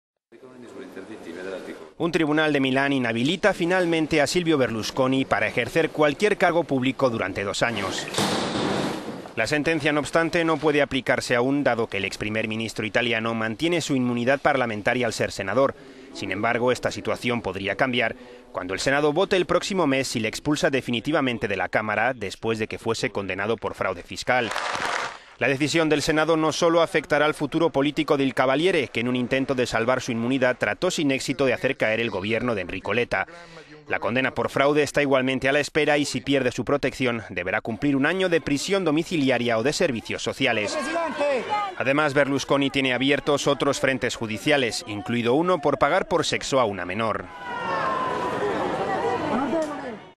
Un tribunal de Milán inhabilita finalmente a Berlusconi para ejercer cualquier cargo público...
La sentencia no obstante no puede aplicarse aún, dado que el ex primer ministro italiano mantiene...

La sentencia no obstante no puede aplicarse aún, dado que el ex primer ministro italiano mantiene su inmunidad parlamentaria al ser senador. Sin embargo, esta situación podría cambiar cuando el Senado vote el próximo mes si le expulsa definitivamente de la cámara, después de que fuese condenado por fraude fiscal en agosto.

La decisión del Senado no sólo afectará al futuro político de "il Cavaliere", que en un intento de salvar su inmunidad trató sin éxito de hacer caer el gobierno de Enrico Letta y provocar elecciones anticipadas. La  condena por fraude está igualmente a la espera y si pierde su protección, deberá cumplir un año de prisión domiciliaria o de servicios sociales.